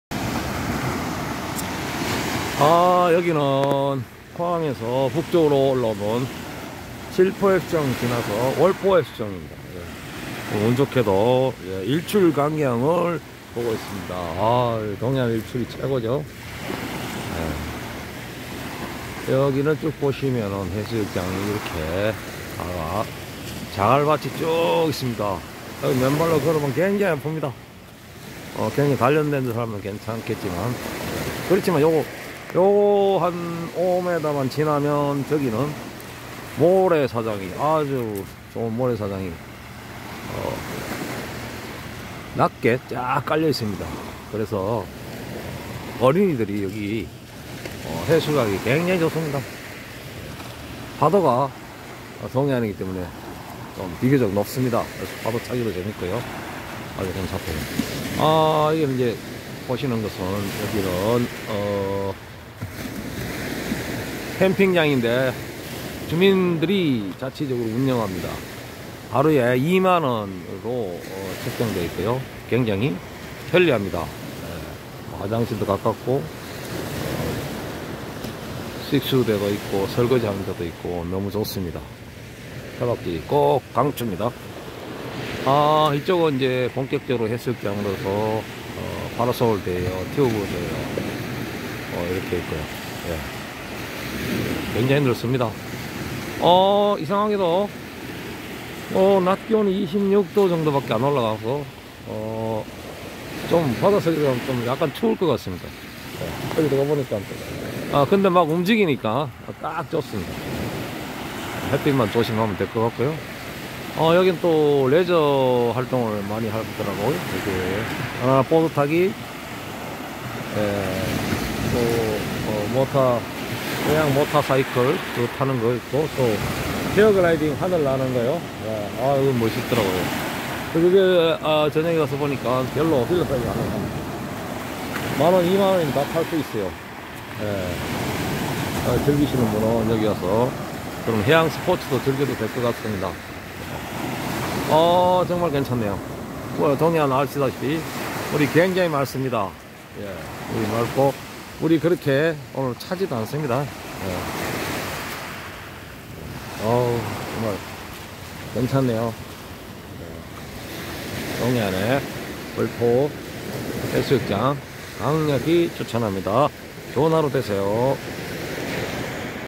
붕어섬 59 Carp island ㅡ 월포해수욕장 차박지 Car camping place at the Wolpo beach
포항에서 북쪽으로 30분 영덕남쪽
월포해수욕장 차박캠핑장: 2만원 화장실, 식수대 완비 바가지 단속함으로
실비 레저활동 ㅡ 제트스키, 바나나보토, 페어글라이딩 등